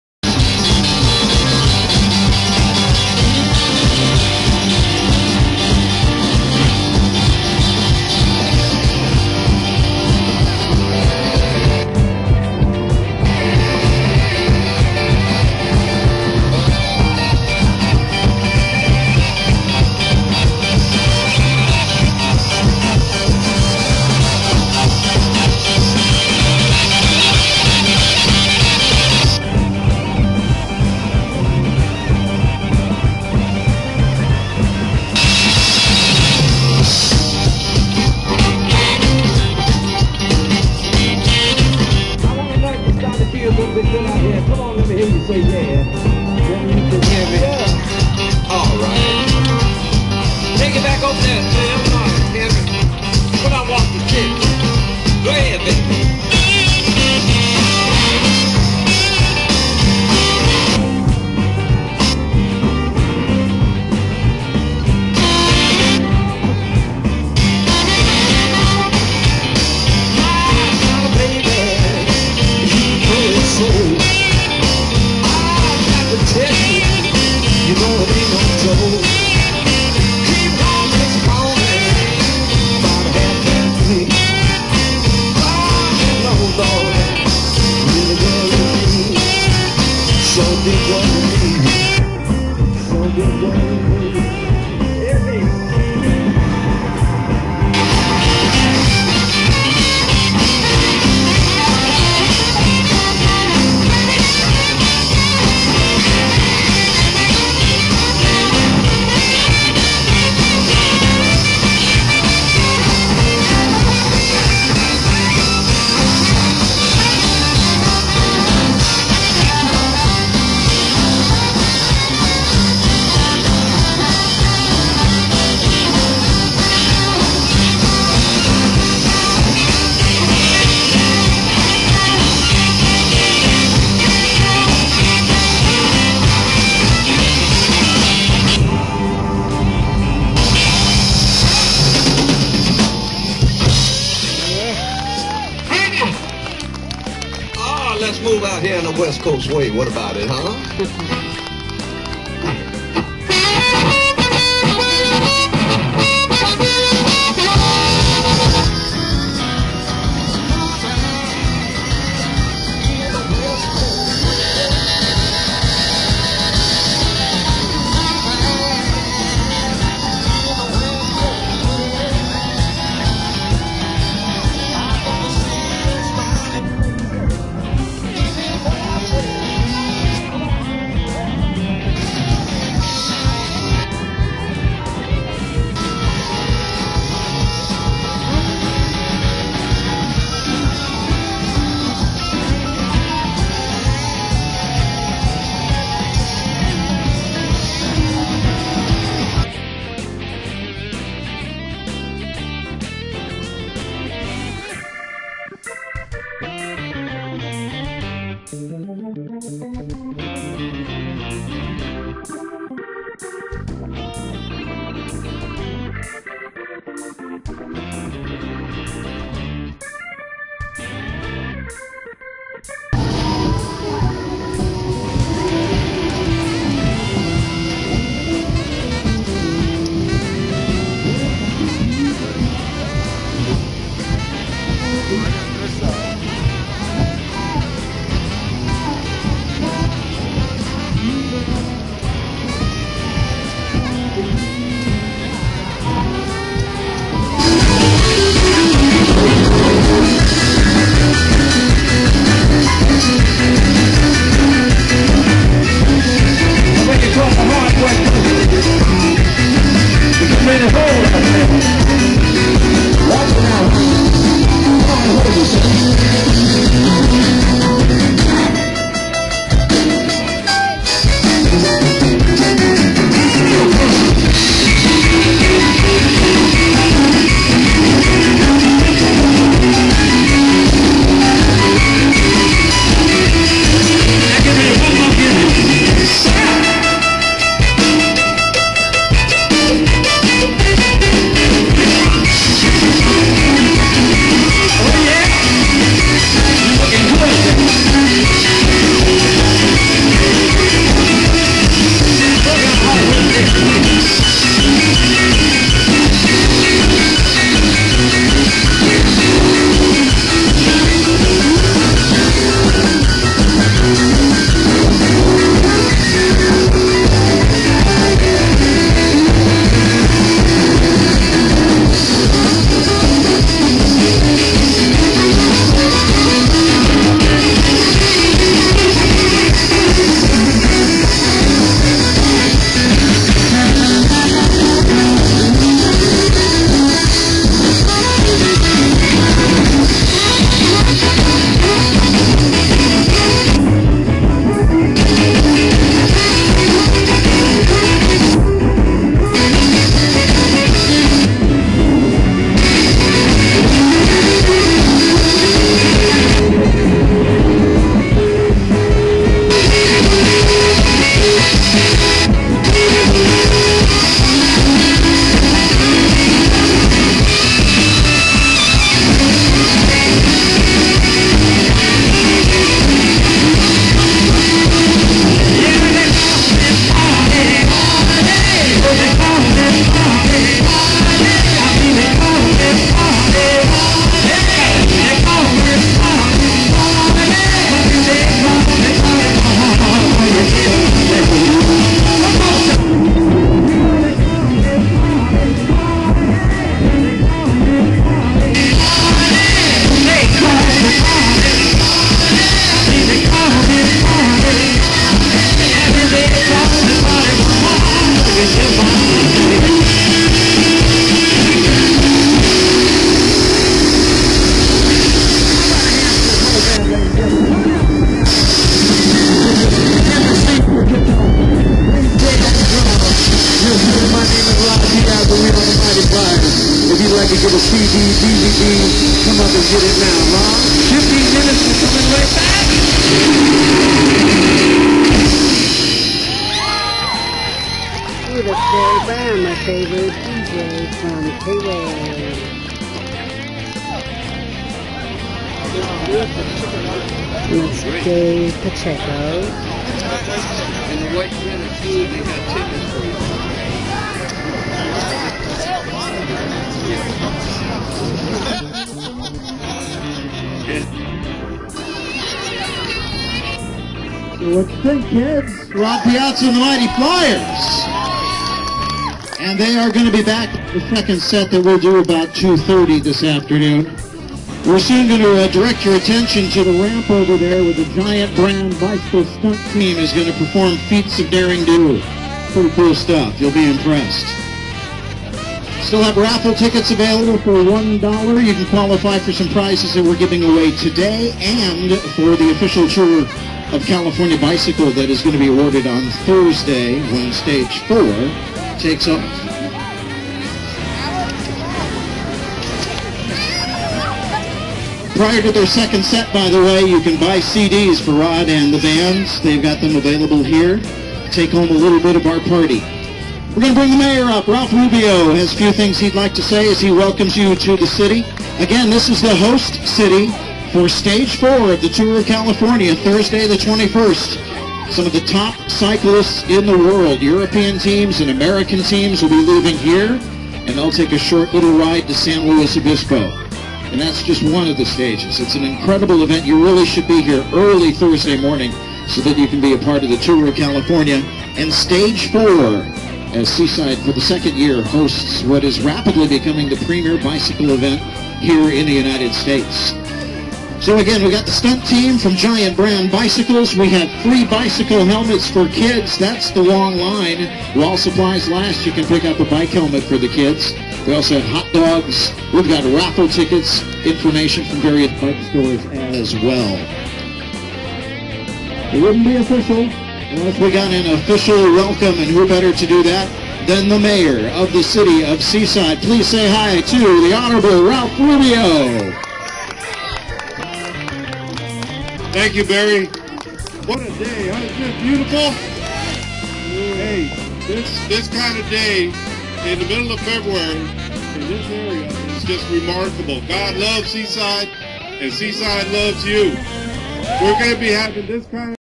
AmgenTour 08 Community Celebration - Part I
A community rally of Central Coast CA City of Seaside to honor their sponsorship of the AMGEN TOUR, Stage 4, on February 10, 2008.  This year, the honor goes to Central Coast CA in Santa Cruz.